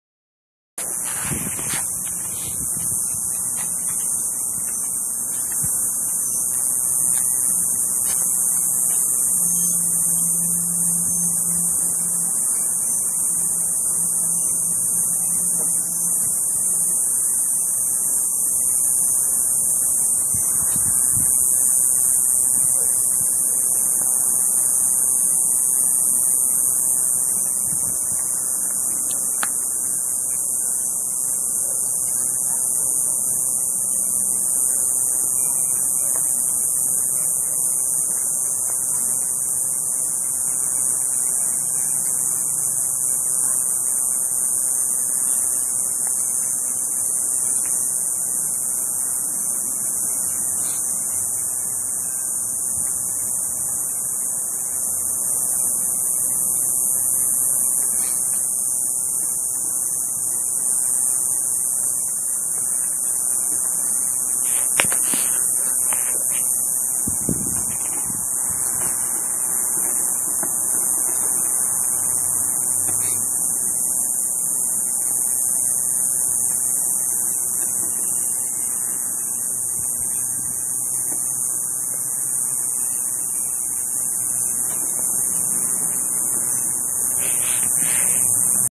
Alien communication in a bunch of Cicadas "LISTEN"
Cicada communication digitally remastered to bring out amazing communication techniques "TURN IT UP LOUD & LISTEN"